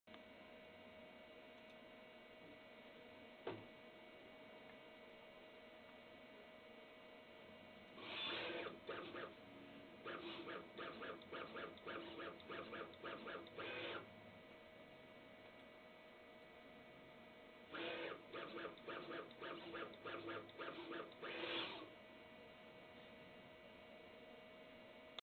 Janome JR3000 Automated Stylus Touch Screen Tester
Using only string commands over Ethernet, robot coordinate locations can be sent dynamically. This program was written in Python. An incorrect passcode is entered then the correct passcode is entered.